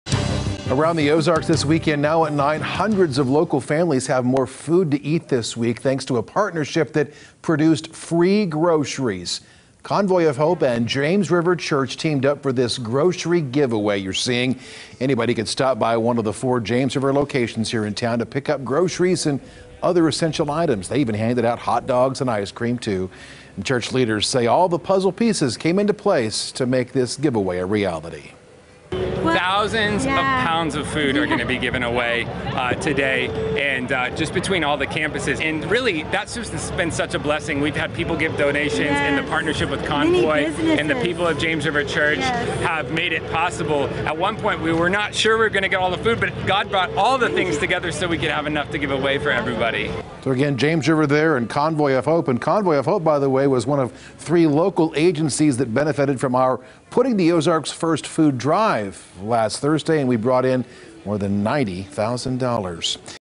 Convoy of Hope, James River Church feed hundreds of families during grocery giveaway
SPRINGFIELD, Mo. -- Hundreds of local families have more food to eat this week thanks to a partnership that produced free groceries.

Convoy of Hope and James River Church teamed up for this grocery giveaway.

Anyone could stop by one of the four James River locations to pick up groceries and other essential items.

They even handed out hot dogs and ice cream.

Church leaders say all the puzzle pieces fell into place to make this giveaway a reality.

Brandon Lindell, Executive Ministeries Paster
"Thousands of pounds of food are going to be given away today, and just between all the campuses," said Brandon Lindell, Executive Ministries pastor. "Really thats been such a blessing. Many businesses, and the people with James River Church have made it possible. At one point, we weren't sure we were going to get all the food, but God broght all the things together so we could have enough to givaway for everybody."

Convoy of Hope was one of three local agencies that benefited by KOLR's "Putting the Ozarks First" food and money drive last Thursday that brought in more than $90,000 dollars.